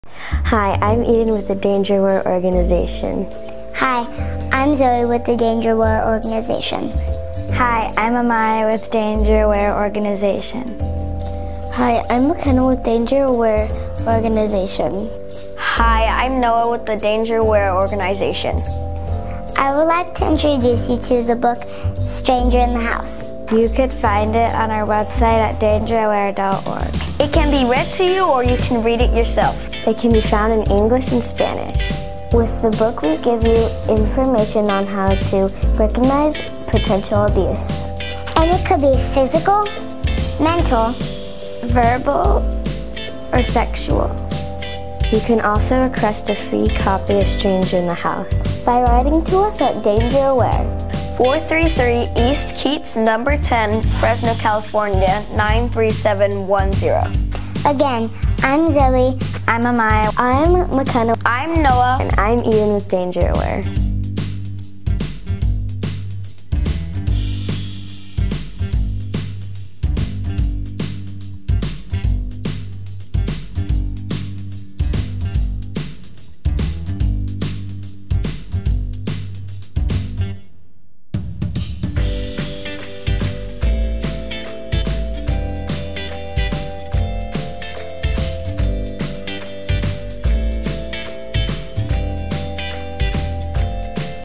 Introducing Stranger In the House
The first book by CEO of Danger Aware Lorilla Fonda.

Help us Help others by Donating.

Danger Aware is also hosting Coloring, Poster, Video and Essay Contest!